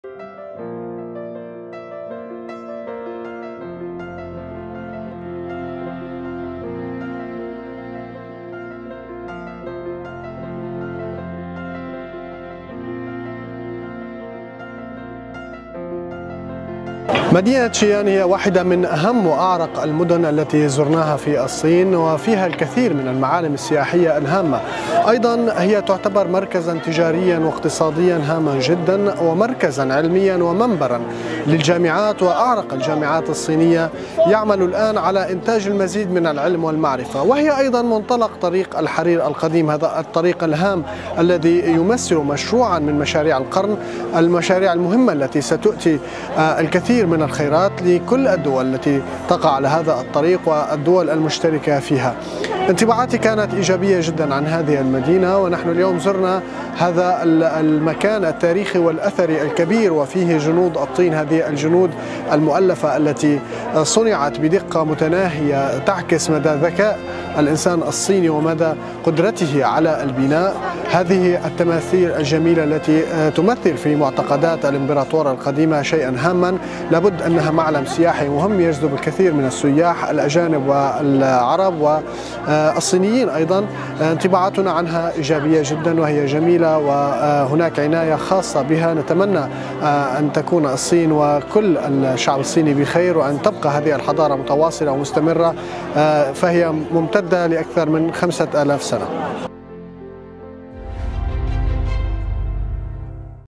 GLOBALink | Syrian journalist expresses love for China's Xi'an
A Syrian journalist attending the just-concluded Silk Road (Xi'an) International Communication Forum has expressed his love for the ancient city of Xi'an.